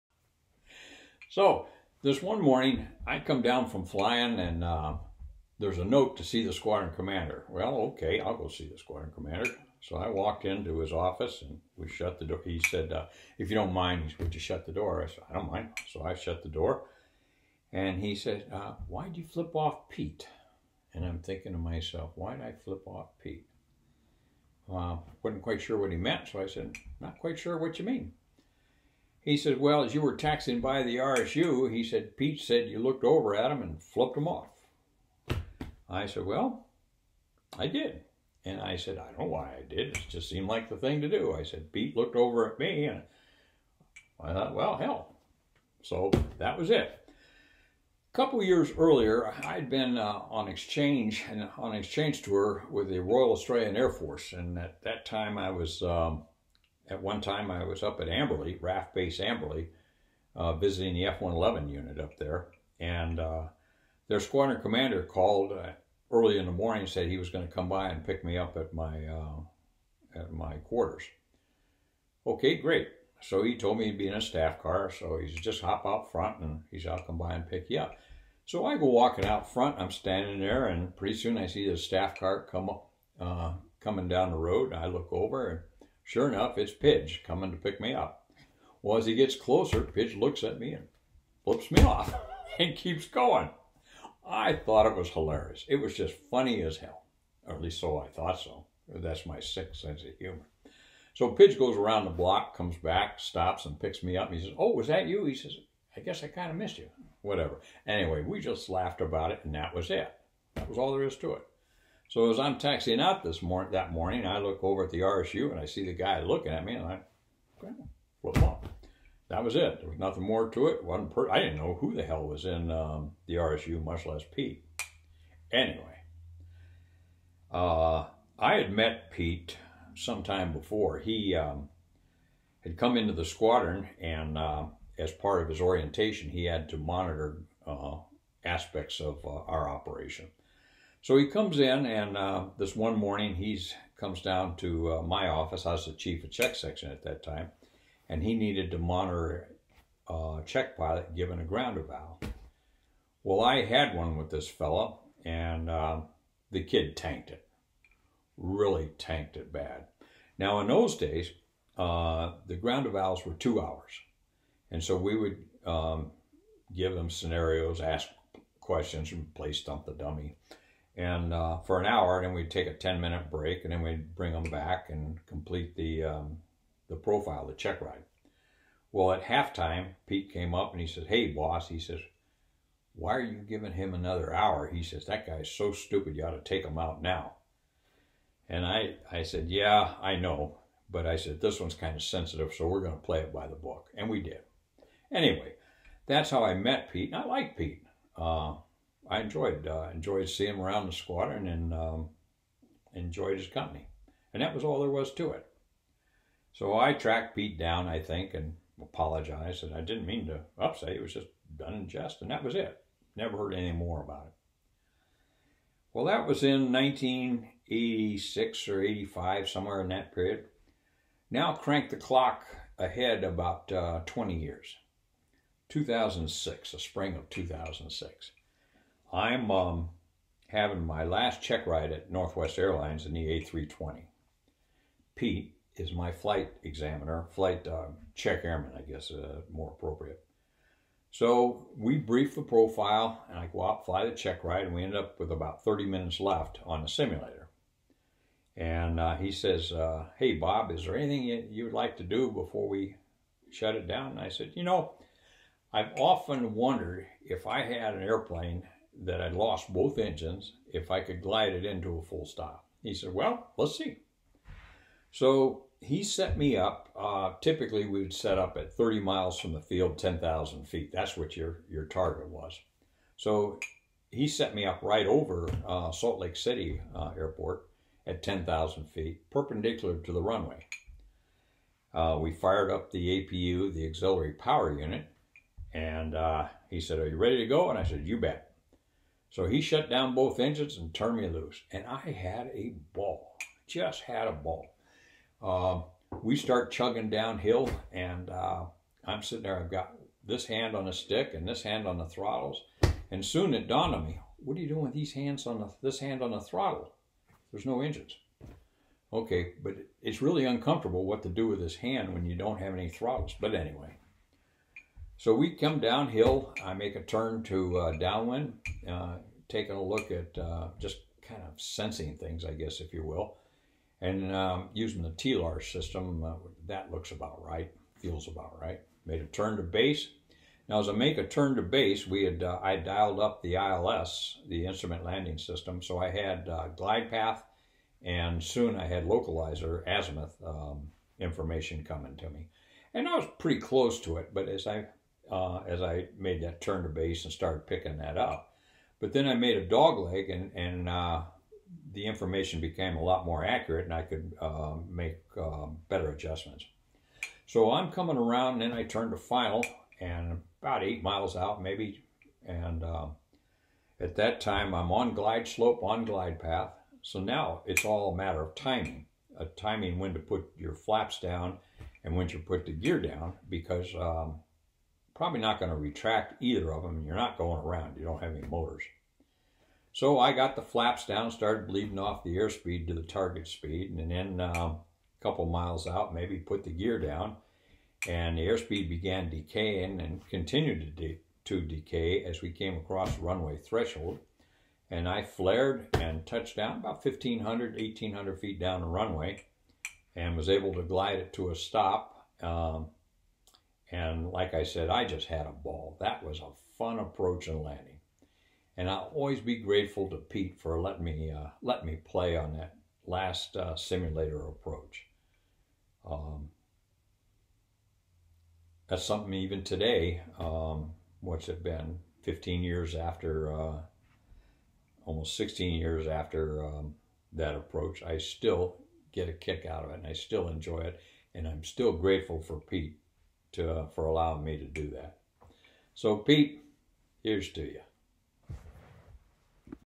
Flipping Off Pete
Pete was in the RSU, the "Little House on the Prairie" one morning when I taxied by.  I had no idea who was in there that day, it was a bright sunny day and I was feeling very good - about to go and fly.  Pete looked over at me, and I don't know why, I just "flipped him off!"  It seemed like the thing to do, until I returned to the squadron.